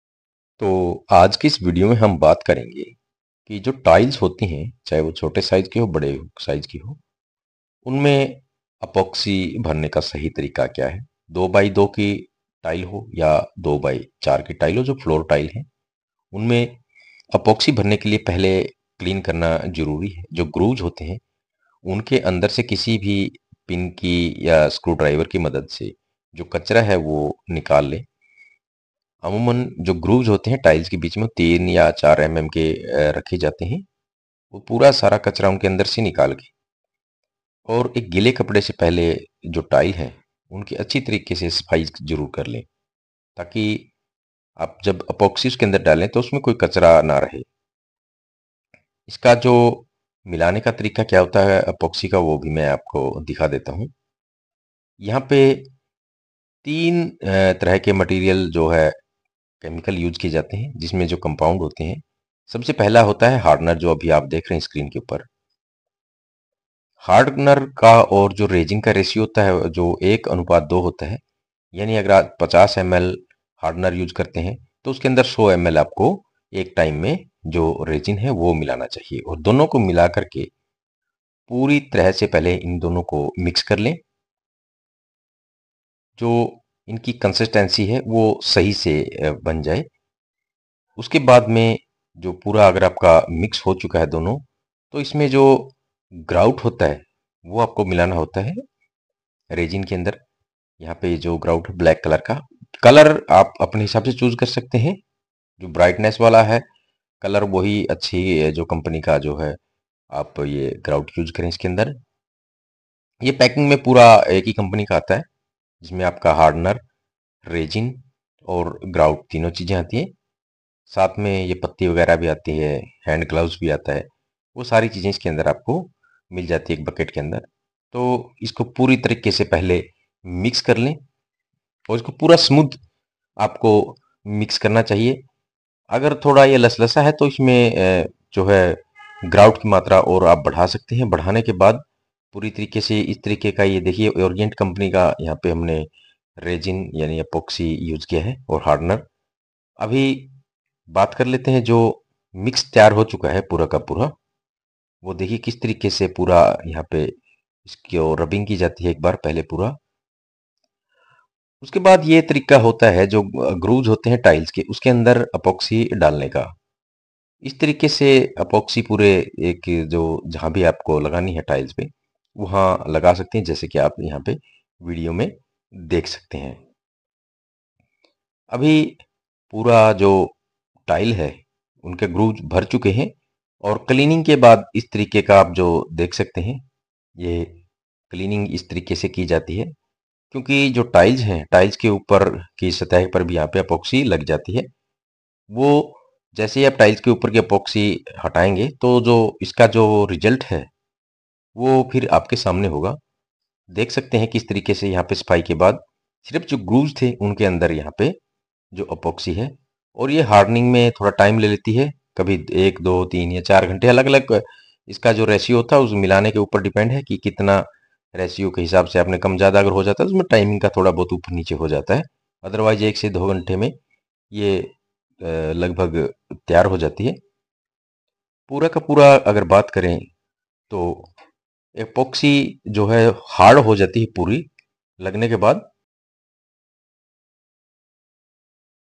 टाइलों में अपोक्सि भरने का सही तरीका। Apoxy Resin Feeling Method in Tile Grooves.
साथियों आप मे से बहुत से लोगों को ये नहीं पता होगा कि जब आपके घर मे टाइल लग जाती है तो उनमें अपोक्सि भरने का सही तरीका क्या है। हमारे इस वीडियो के माध्यम से आप जरूर सिख जाएंगे कि सही तरीका क्या है।